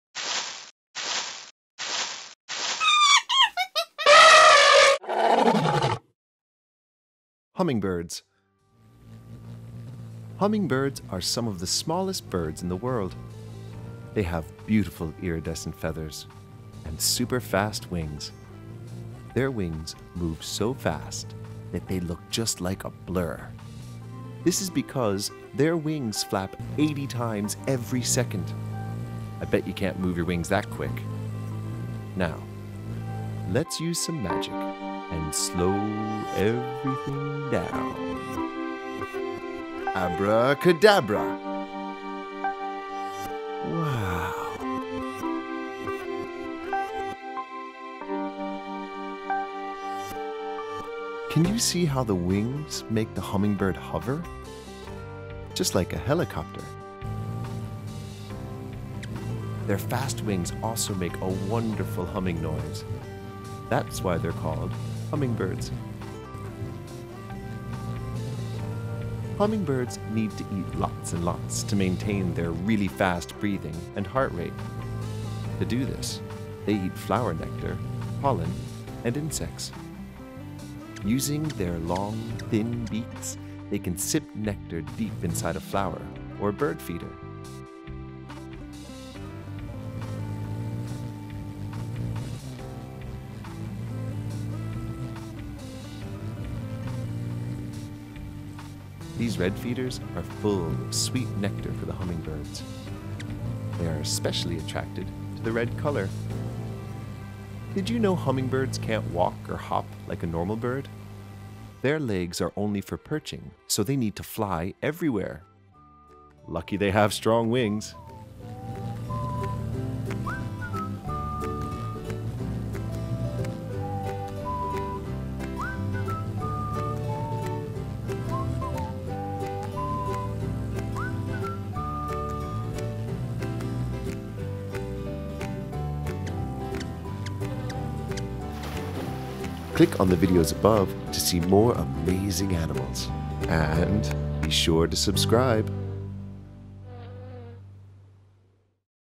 * HUMMINGBIRD * | Animals For Kids | All Things Animal TV
* HUMMINGBIRD * | Animals For Kids
Quality, educational videos for kids. Made in the UK.

Hummingbirds are some of the smallest birds in the world.

They have beautiful iridescent feathers. And super fast wings.

Their wings move so fast… that they look just like a blur. This is because their wings flap 80 times every second. I bet you can’t move your arms that quick.

Now… Let’s use some magic and slooow everything down…!
Abracadabra! Wow. Can you see how the wings make the hummingbird hover? Just like a helicopter. Their fast wings also make a wonderful humming noise. That’s why they are called hummingbirds.

Hummingbirds need to eat lots and lots, to maintain their really fast breathing and heart rate. To do this they eat flower nectar, pollen and insects. Using their long thin beaks they can sip nectar deep inside a flower or bird feeder. These red feeders are full sweet nectar for the hummingbirds. They are especially attracted to the red colour.

Did you know hummingbirds can’t walk or hop like a normal bird.
There legs are only for perching so they need to fly everywhere.
Lucky they have strong wings.